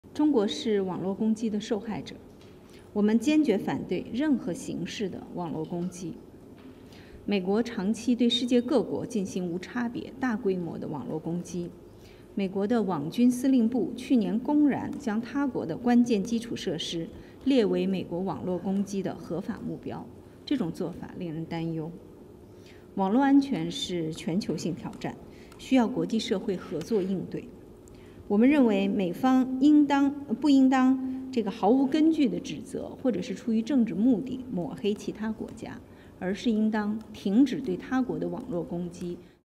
Beijing: US targets critical infrastructure of other countries but plays the victim on cyber attacks
Bloomberg: A US energy executive has told lawmakers in the US that China represents an increasing threat to the US power grid. The executive also called China’s activities regarding cyber hacking quite alarming. How does the foreign ministry respond?

Mao Ning: China is a victim of cyber attacks. We firmly oppose any form of cyber attacks. The US has carried out indiscriminatory, large-scale cyber attacks against other countries over the years. The US Cyber Force Command blatantly declared last year that the critical infrastructure of other countries is a legitimate target for US cyber attacks. Such moves have raised concern.

Cyber security is a global challenge and requires joint response from the international community. Some in the US must not smear other countries with groundless narratives to serve their political agenda. The US needs to stop cyber attacks against other countries.